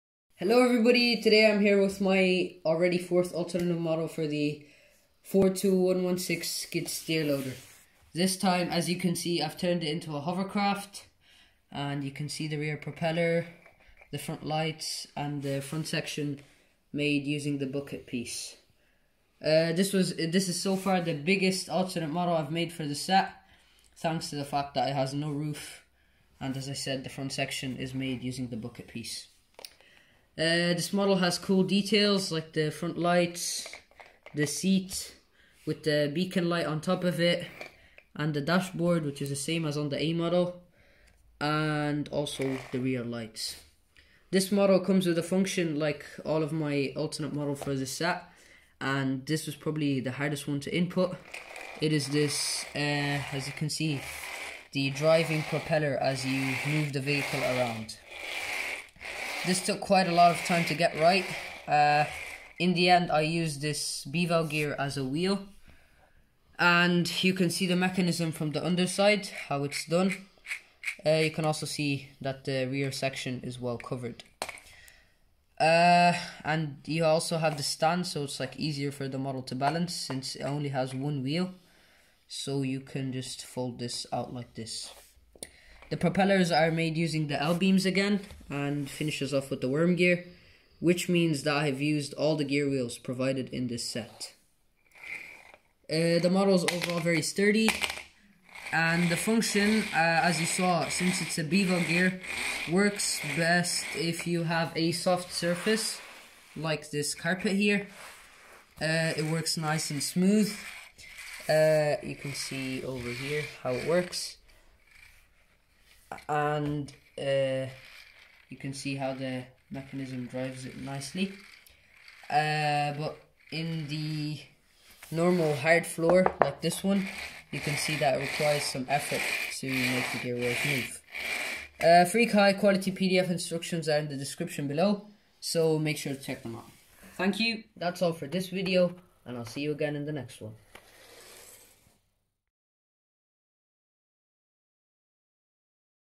Lego Technic 42116 Alternate 4/F model - Hovercraft with FREE pdf instructions!!
Hello everybody, I hope you are doing well. Today I am here with my fourth alternate model for the 42116 Skid Steer Loader, this time I have turned it into a hovercraft! Of course there are some slight differences between this and the real vehicle but considering the scale, I am pretty happy with the looks of the model, especially the use of the bucket piece and the  seat detail! The model's only function is that when you push it forward, the rear propeller rotates